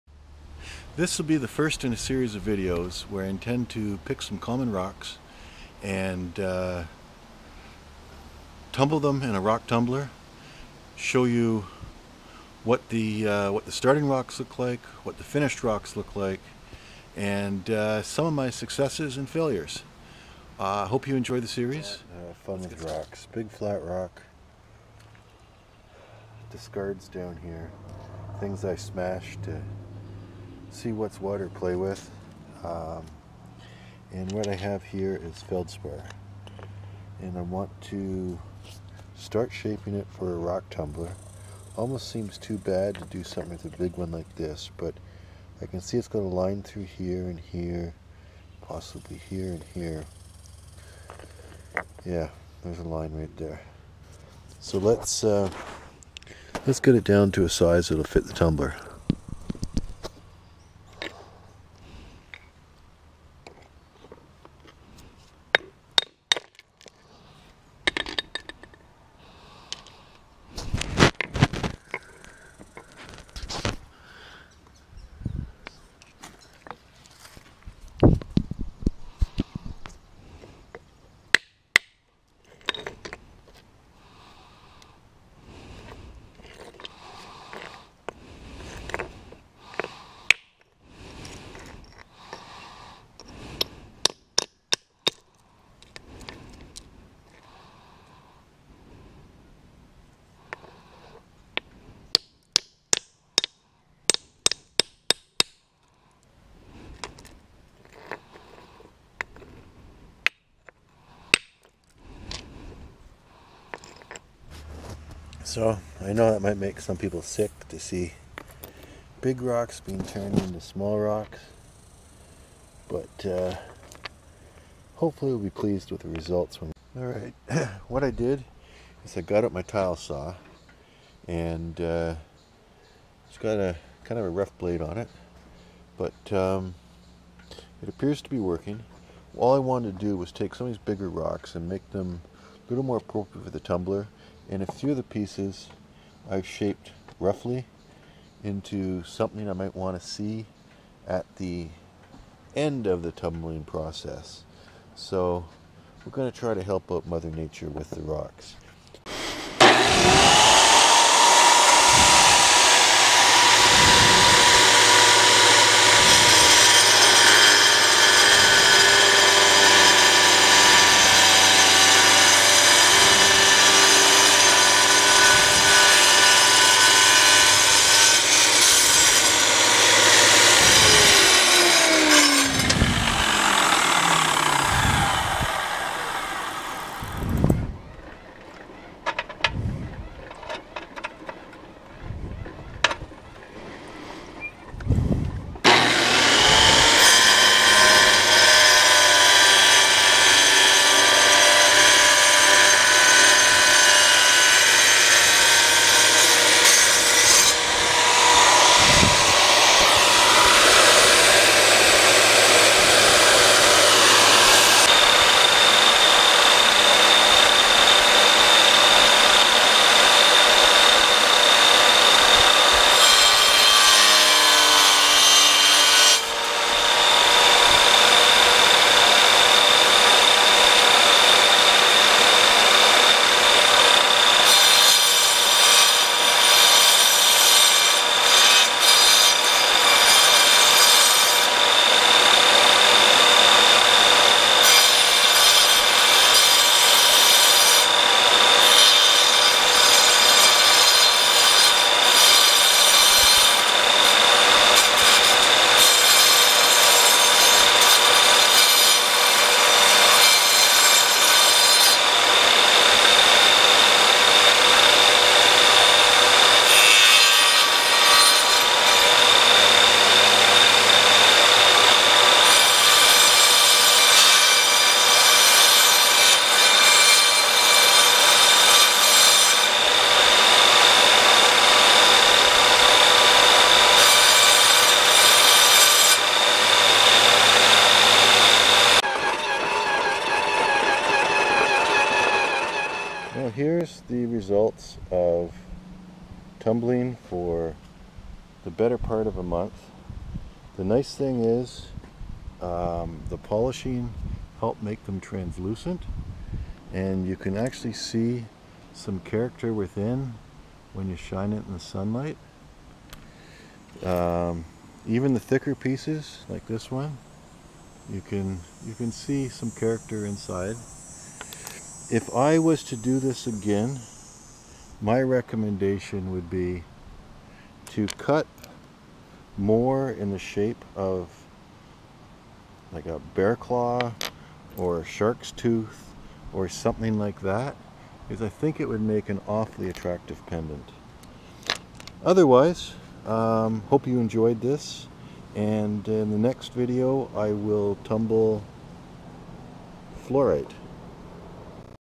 Preparing & Rock Tumbling K-Feldspar
Before and After. Breaking and shaping Potassium Feldspar for polishing in a rock tumbler.